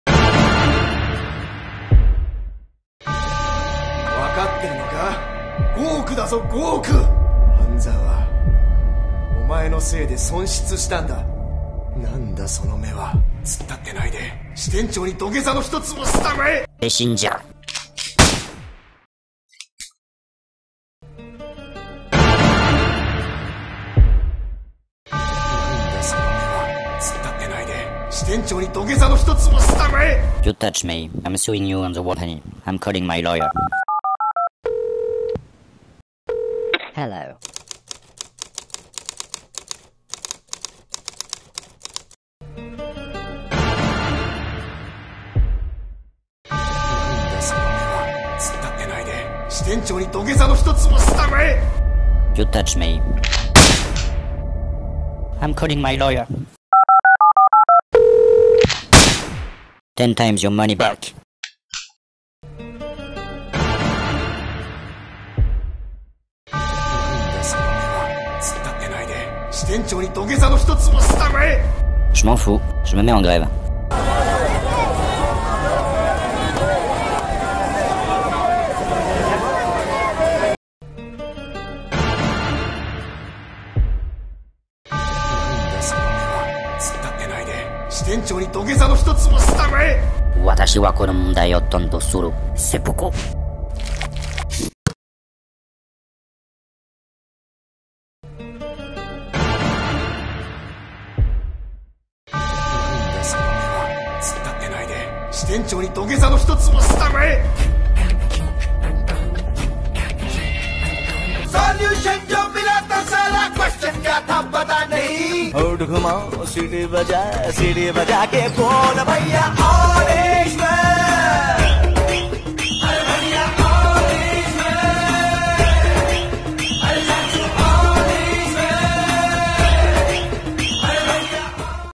如果"半澤直樹"不是日本人... (Thomas B. #01)
...故事不會那麼長.

謝謝您的支持~

"If Hanzawa Naoki wasn't Japanese... the story wouldn't be so long..."

Summary:
Naoki Hanzawa joined the Tokyo Chuo Bank and becomes the head of the loans division at the Osaka Nishi branch. He is forced by branch manager Asano to grant an unsecured loan of 500 million yen to Nishi Osaka Steel. Source :Wikipedia

大家好...
我沒想到這個影片會那麼收歡迎. 謝謝你們 !